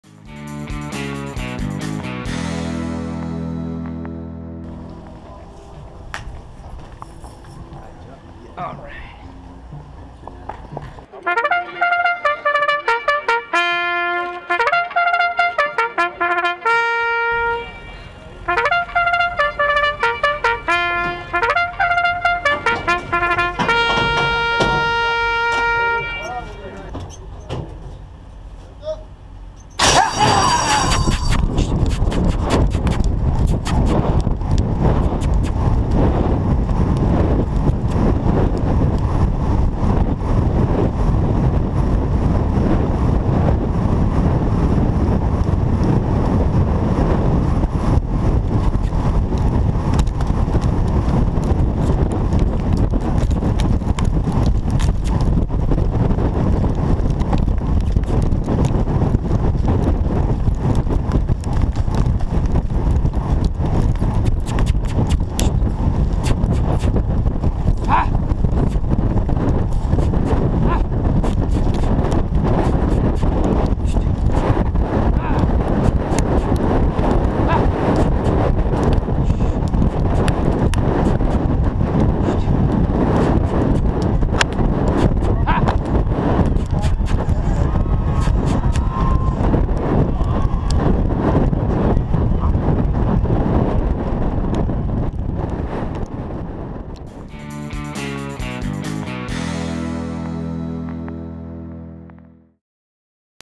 Jockey Cam: GoPro Camera on Horse Jockey
For the first time at Lone Star Park a GoPro was mounted on the helmet of a jockey. Roman Chapa shows us what it is like to be on a thoroughbred during a live race. This is the best behind the scenes video ever at LoneStarPark.